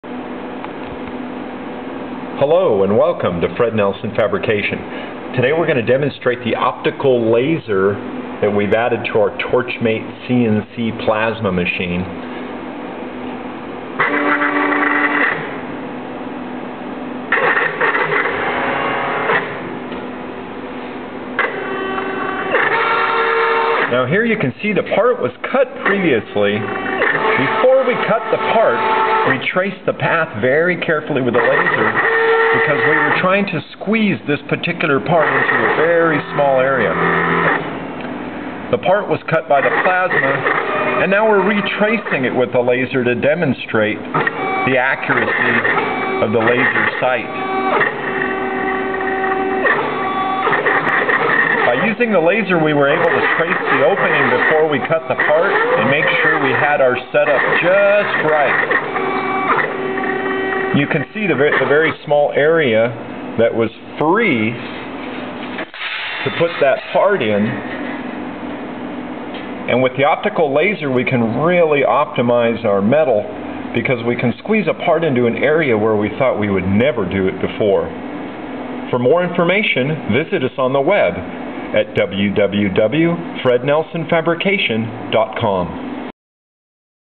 Optical Laser Demonstration
Produced by FredNelsonFabricaion.com
Post cutting demonstration of an optical laser sight on a Torchmate CNC Plasma Machine. Plasma cutting torch is tool one, optical laser is tool two, Plate Marker (engraver) is tool three.